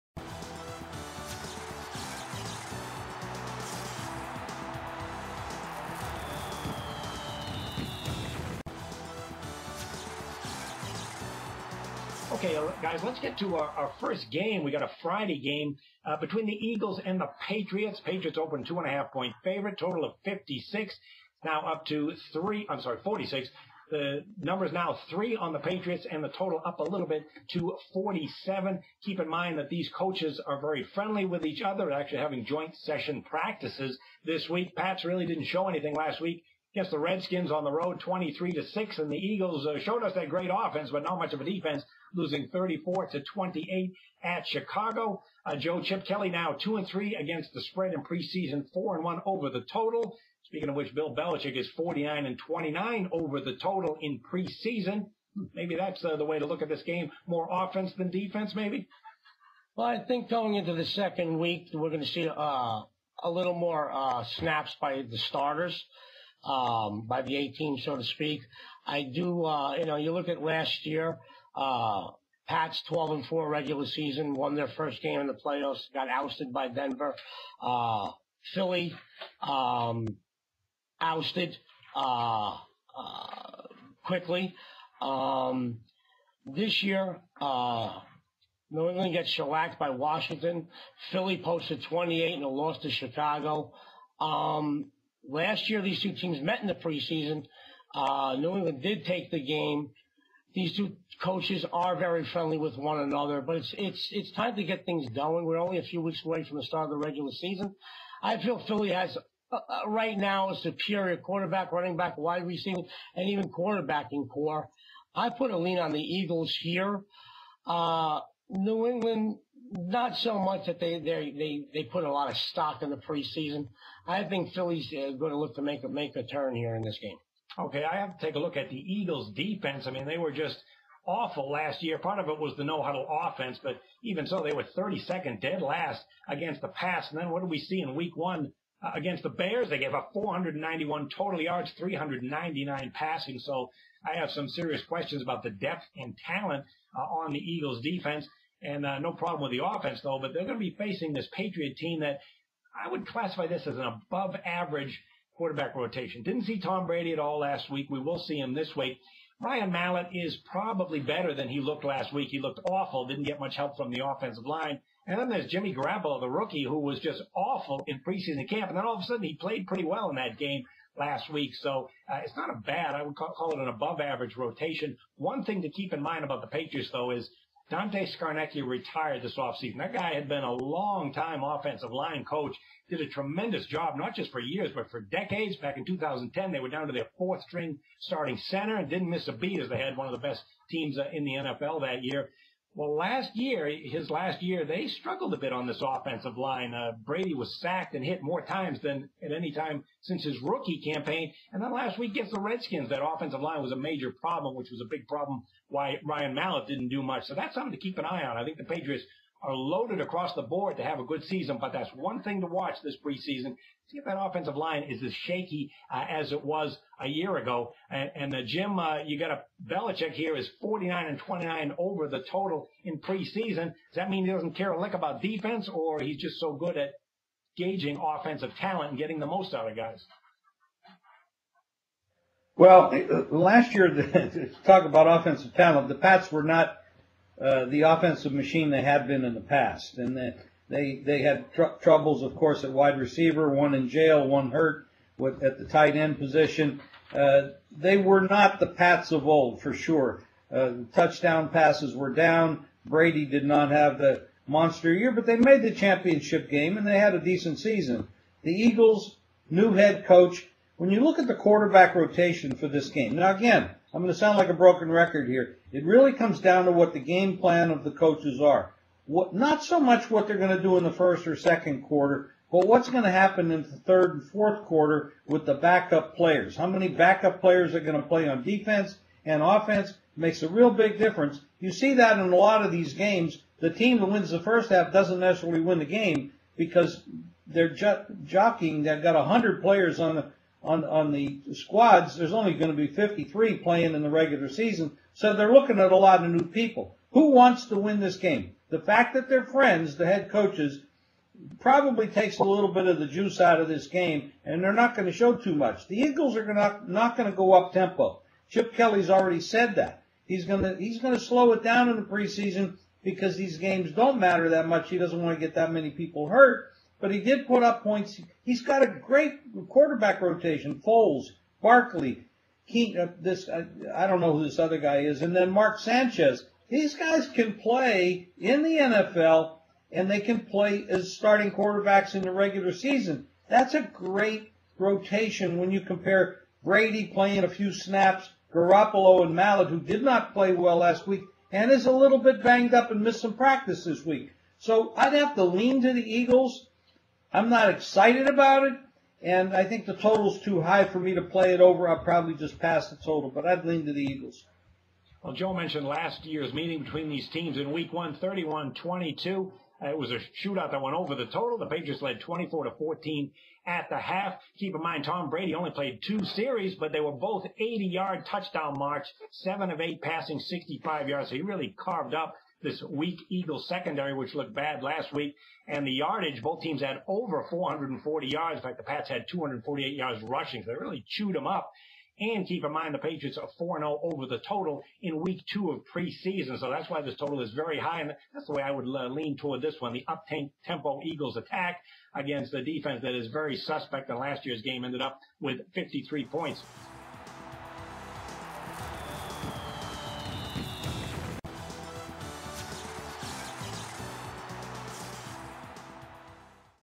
Philadelphia Eagles vs. New England Patriots Betting Preview, August 15, 2014
Week 2 of the NFL preseason has the Philadelphia Eagles vs. New England Patriots and Las Vegas handicappers Jim Feist and Joe D'Amico examine the game, the point spread and the total. Is home field and Tom Brady enough? Or is the wawgering edge with the underdog Eagles, or the total?

"Philadelphia Eagles vs. New England Patriots" "NFL" "odds" "las vegas" "betting" "Tom Brady"